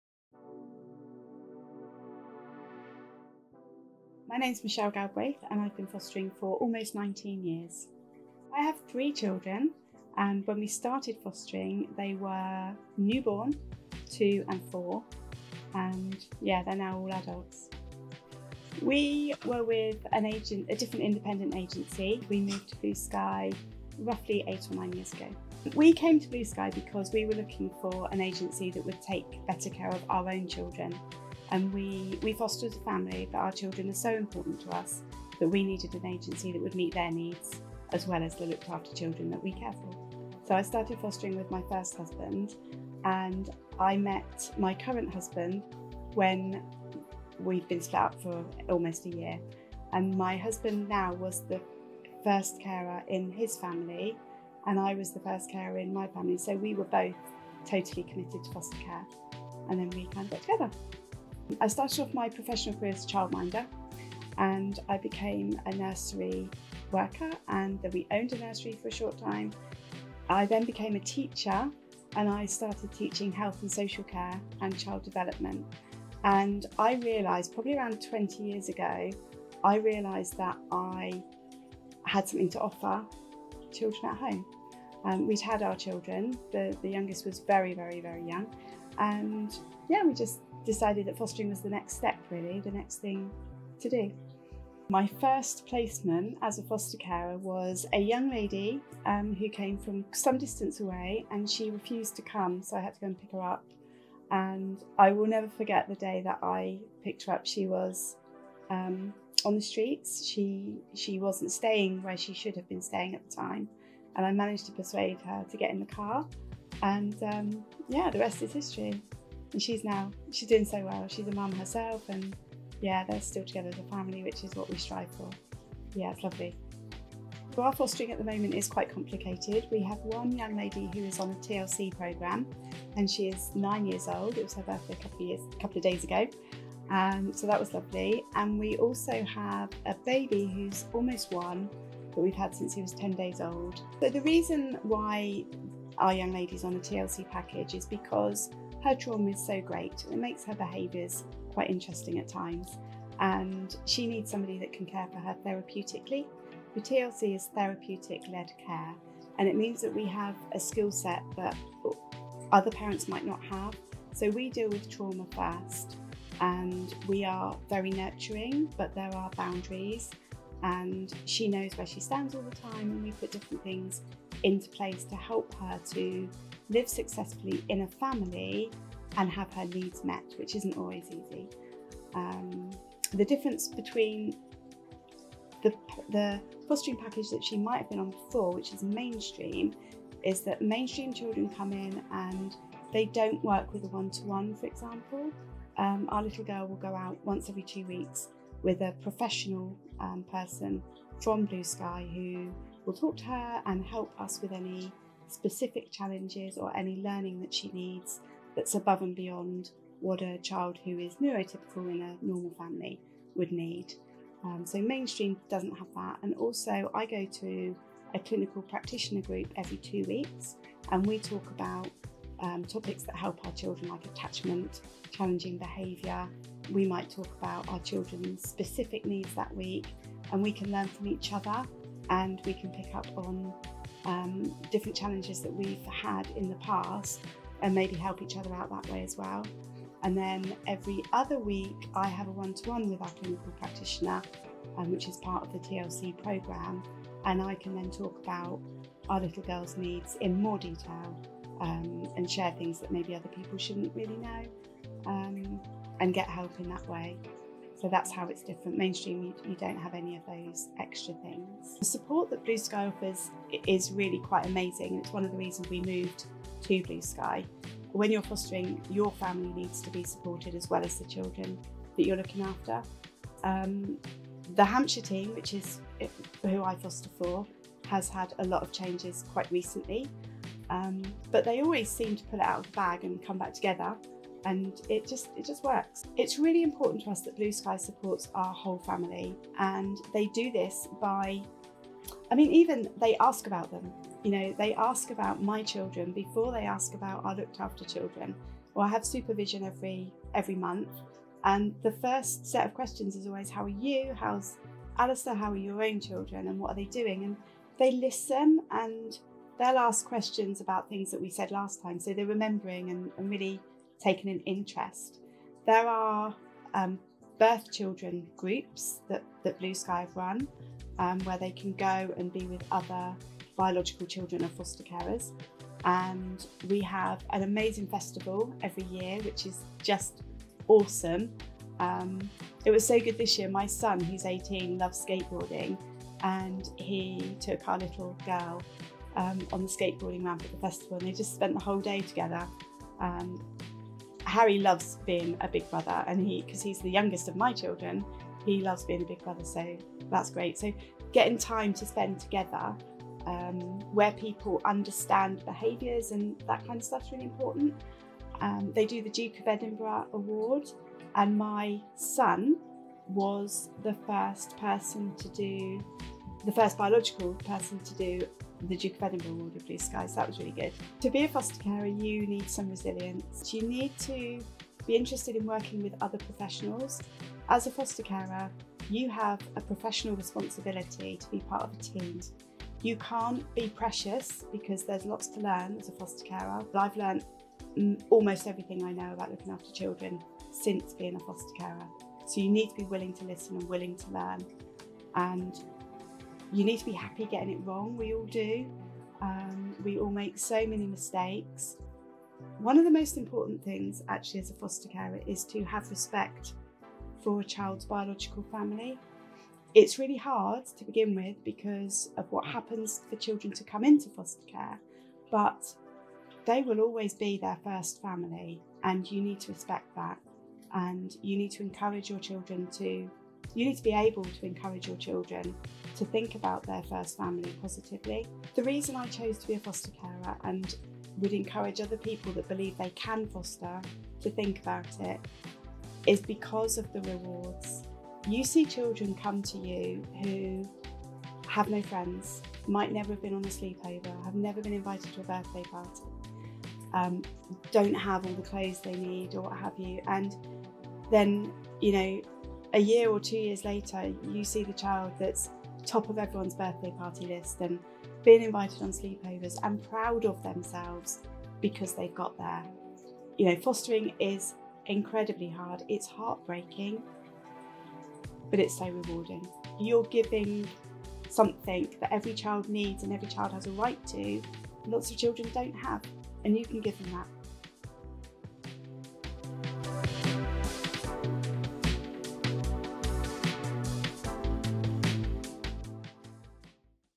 Michelle Galbraith: Therapeutic Led Care Fostering
Michelle Galbraith describes her experience as a Therapeutic Led Care foster carer with Blue Sky Fostering.